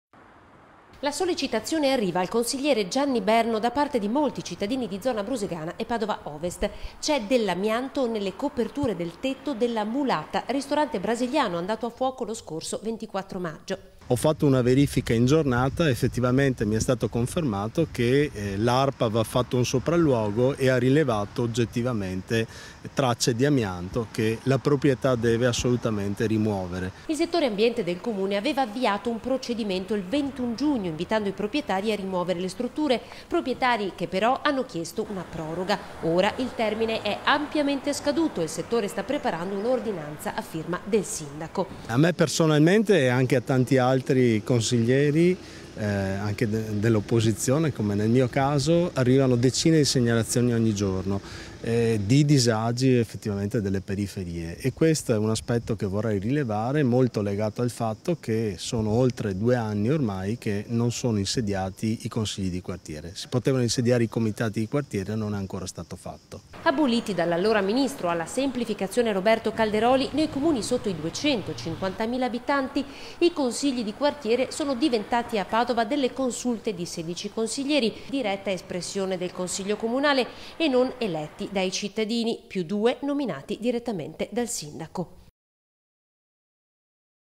TG PADOVA (23/08/2016) - AMIANTO ALLA “MULATA”: CITTADINI CHIEDONO INTERVENTI SUBITO
TG PADOVA (martedì 23 agosto 2016) - La sollecitazione arriva al consigliere Gianni Berno da parte di molti cittadini di zona Brusegana e Padova Ovest: c’è dell’amianto nelle coperture del tetto della Mulata, ristorante brasiliano andato a fuoco lo scorso 24 Maggio
Il settore Ambiente del Comune aveva avviato un procedimento il 21 Giugno invitando i proprietari a rimuovere le strutture, proprietari che, però,  hanno chiesto una proroga. Ora il termine è ampiamente  scaduto  il Settore sta preparando un'ordinanza a firma del Sindaco.
Aboliti dall’allora ministro alla Semplificazione Roberto Calderoli, nei comuni sotto i 250000abitanti, i consigli di Quartiere, sono diventati a Padova, consulte di 16 consiglieri nominati dal consiglio e non direttamente dai cittadini. più due nominati direttamente dal Sindaco.